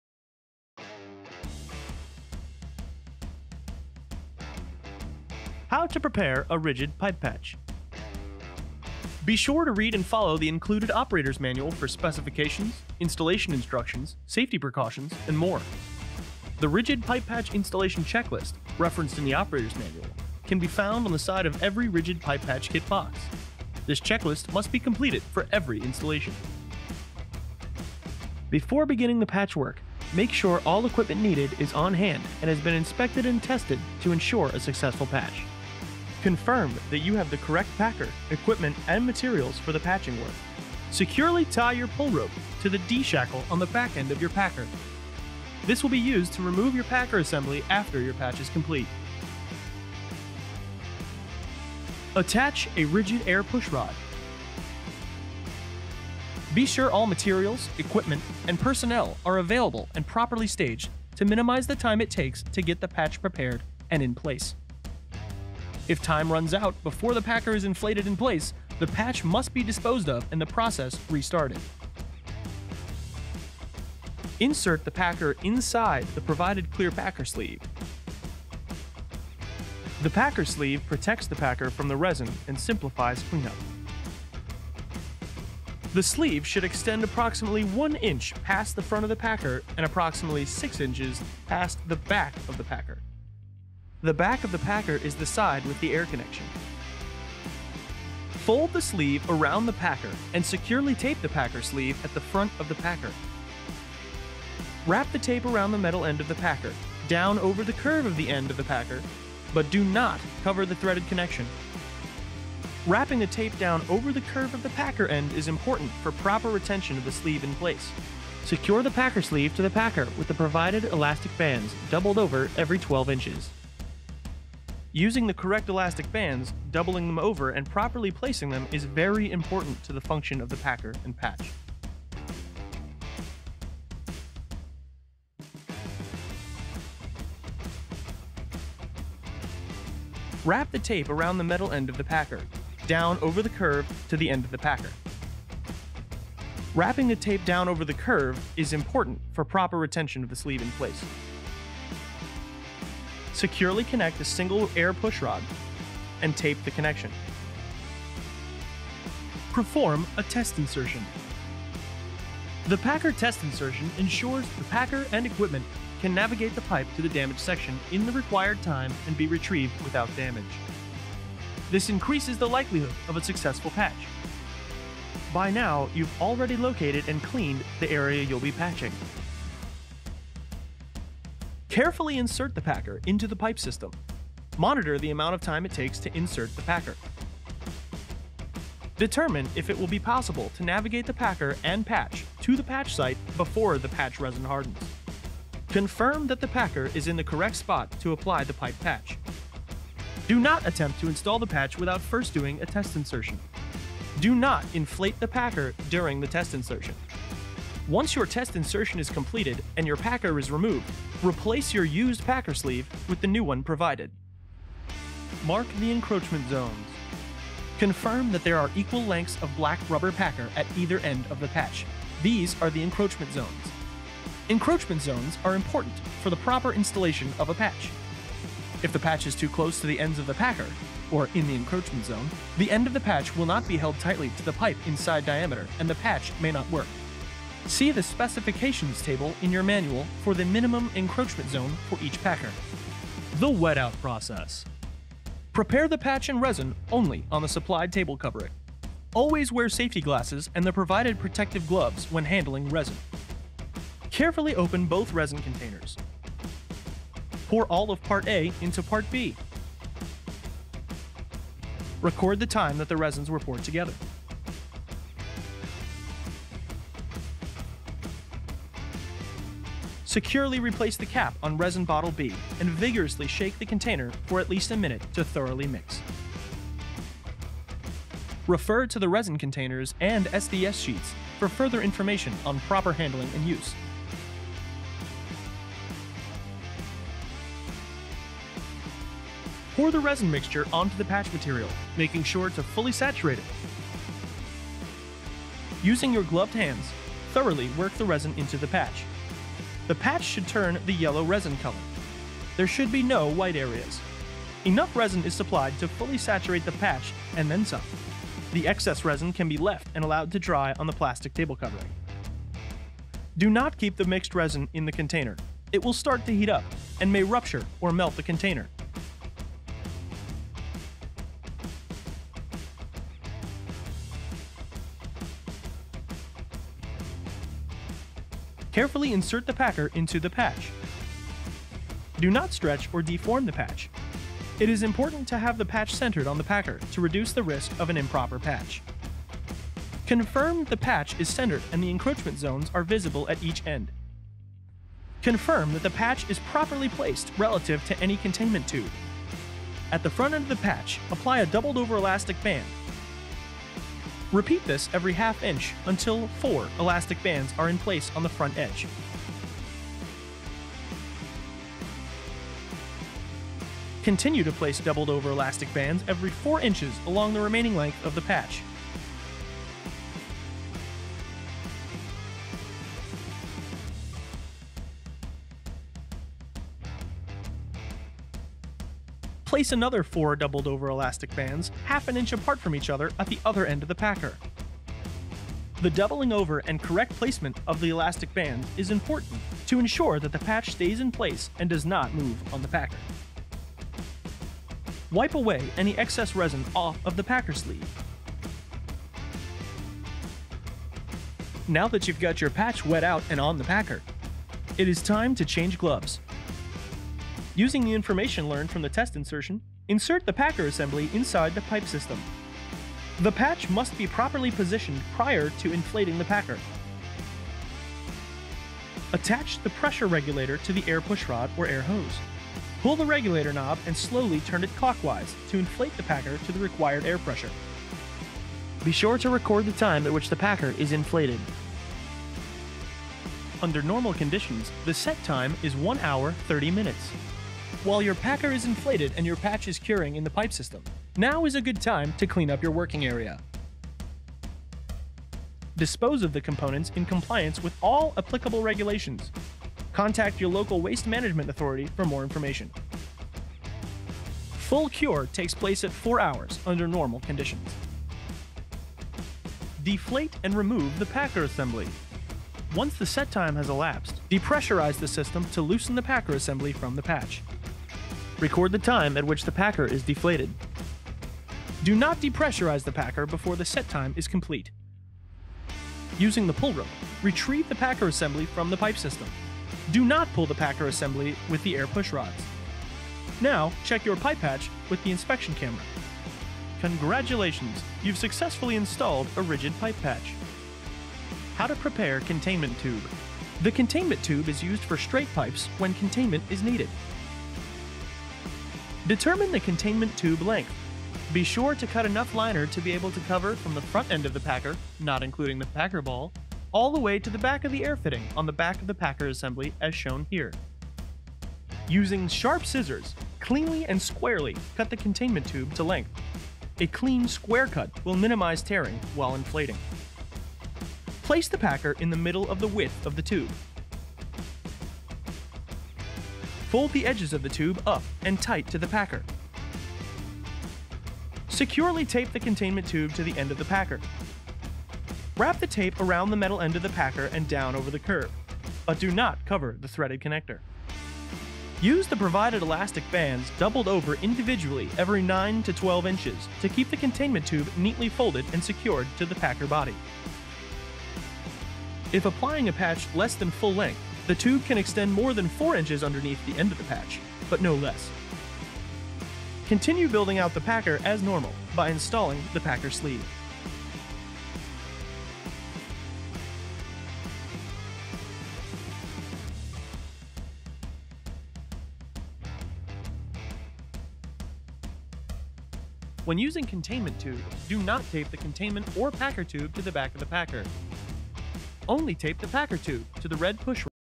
RIDGID® Pipe Patch System Overview
Get your RIDGID Pipe Patching kits at AIS Industrial in Denver, CO! We are an authorized dealer.

Introducing a brand-new category for RIDGID Tools: Pipe Patching. RIDGID Pipe Patching is a trenchless repair solution that introduces new, incremental revenue stream for professionals already participating in drain cleaning and sewer inspections. The RIDGID Pipe Patch Starter System allows you to complete up to 6 feet of trenchless pipe repairs in 2-inch to 6-inch lines.

0:00 – 2:39 – How to Prepare a RIDGID Pipe Patch
2:40 – 3:35 – Perform a Test Insertion
3:36 – 4:03 - Mark the Encroachment Zones
4:04 – 7:39 - The Wet Out Process
7:40 - 8:11 – Deflate and Remove The Packer Assembly
8:12 – 9:51 – How to Prepare Containment Tube
9:52 – 11:18 – How to Prepare Containment Liner
11:19 – Closing Information

Video credit to RIDGID. We are an authorized dealer.